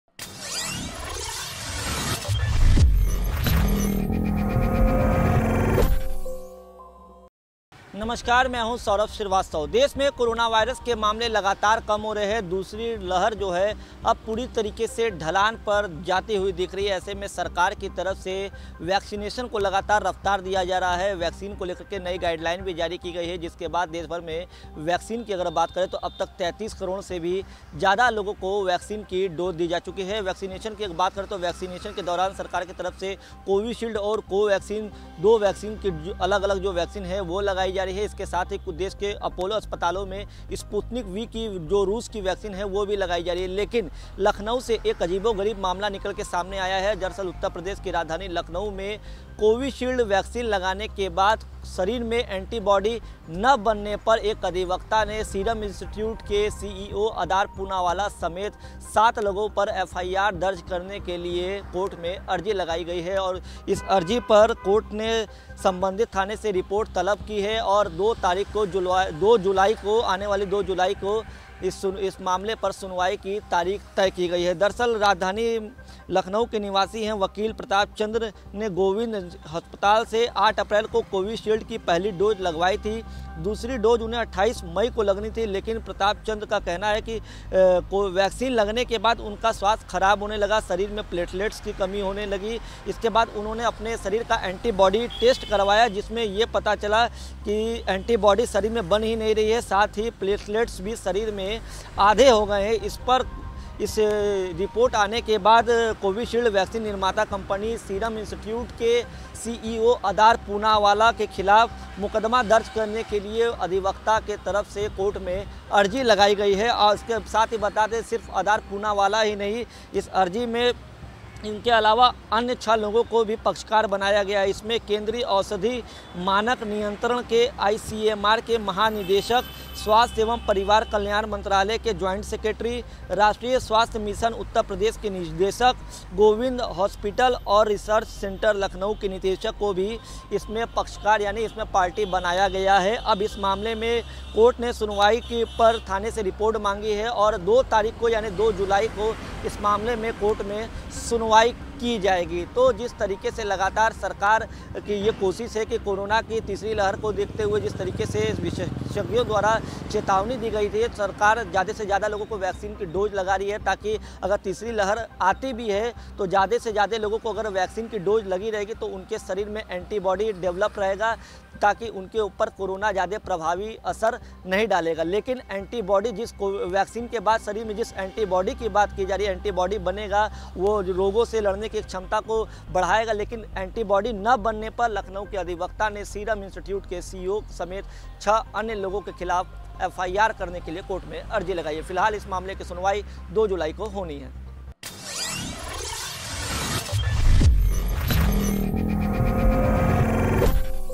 कोविशील्ड लगवाने वाले शख्स ने ADAR POONAWALLA समेत ICMR,WHO के खिलाफ किया केस
कोरोना के काम हो रहे मामलों  के बीच सरकार ने वैक्सिनेशन को रफ्तार दी है। देशभर में  कोविशील्ड, कोवैक्सीन और कुछ अस्पतालों में रूस की स्पूतनिक वी वैक्सीन लगाई जा रही है।  ऐसे में उत्तर प्रदेश से एक हैरान करने वाला मामला सामने आया है। उत्तर प्रदेश की राजधानी लखनऊ में कोविशील्ड वैक्सीन लगवाने के बाद शरीर में एंटीबॉडी ना बनने पर एक वकील ने सीरम इंस्टिट्यूट के सीईओ आदार पूनावाला समेत सात लोगों के खिलाफ मुकदमा दर्ज करने के लिए कोर्ट में अर्जी दी है। जिस पर कोर्ट ने संबंधित थाने से रिपोर्ट तलब करते हुए सुनवाई की तारीख 2 जुलाई तय की है। लखनऊ के अधिवक्ता प्रताप चंद्र ने  गोविंद हॉस्पिटल से कोविशील्ड की पहली डोज लगवाई थी जिसके बाद उनकी तबीयत खराब रहने लगी इसके बाद उन्होंने अपना एंटीबॉडी टेस्ट करवाए तो पता चला कि एंटीबॉडी बनी ही नहीं, साथ ही शरीर में प्लेटलेट्स भी कम हुआ है। इसके बाद अधिवक्ता प्रताप चंद्र को भी कोविशील्ड के निर्माता सीरम इंस्टीट्यूट के सीईओ आदार पुनवाला के साथ छह अन्य लोगो को पक्षकार बनाया है।